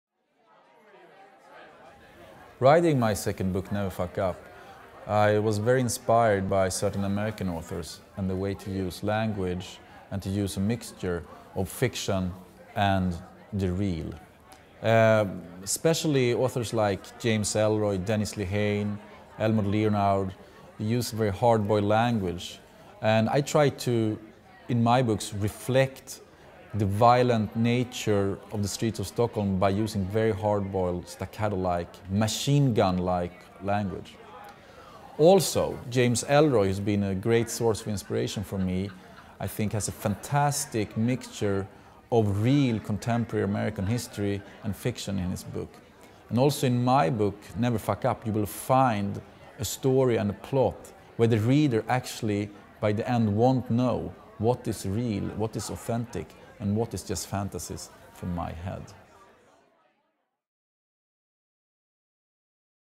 NEVER FUCK UP - Jens Lapidus on writing it
English interview with Jens Lapidus about the second installment in his Stockholm Noir series: NEVER FUCK UP